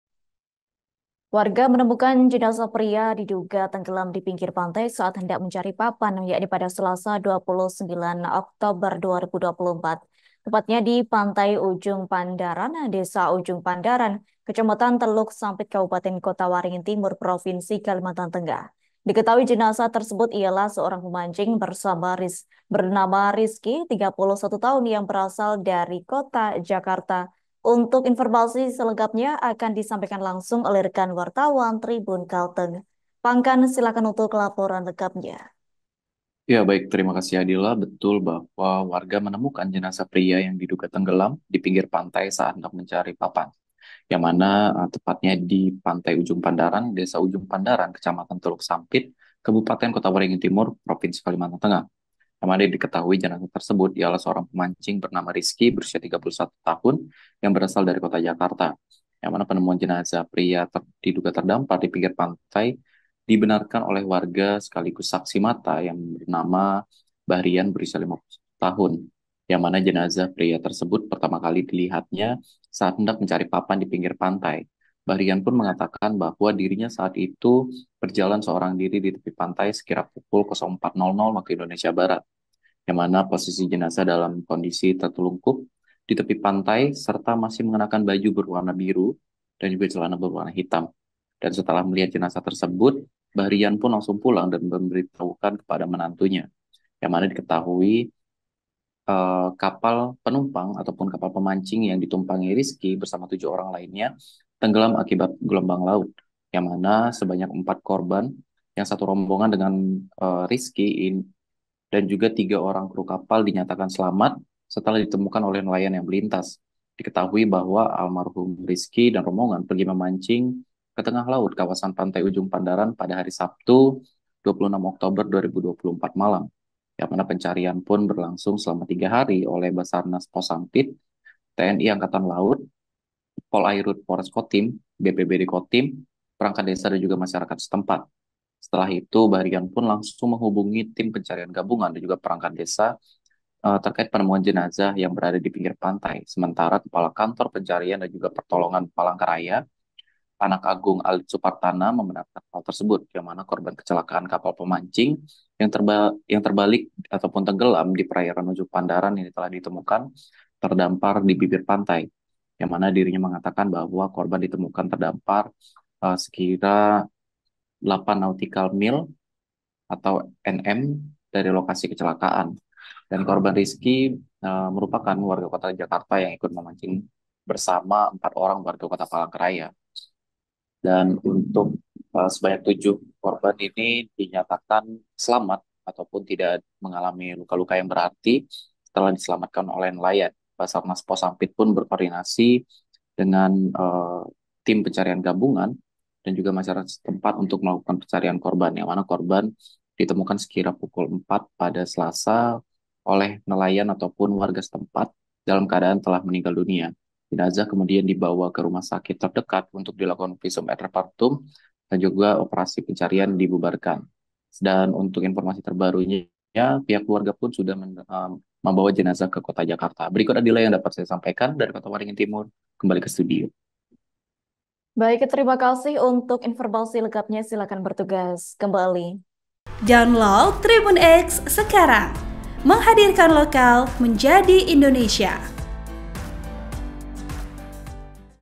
Kesaksian Warga Temukan Jasad Terdampar di Pantai Ujung Pandaran Kalteng, 3 Hari Hilang Tenggelam
TRIBUN-VIDEO.COM-  Warga temukan jenazah pria diduga tenggelam di pinggir pantai saat hendak mencari papan, Selasa (29/10/2024) subuh.

Tepatnya di Pantai Ujung Pandaran, Desa Ujung Pandaran, Kecamatan Teluk Sampit, Kabupaten Kotawaringin Timur, Provinsi Kalimantan Tengah.

Diketahui jenazah tersebut ialah seorang pemancing bernama Rizky (31) yang berasal dari Kota Jakarta. (*)

Program: Live Update
Host: Adila Ulfa Muna Risna
Editor Video: Latif Ghufron Aula
Uploader: Restu Riyawan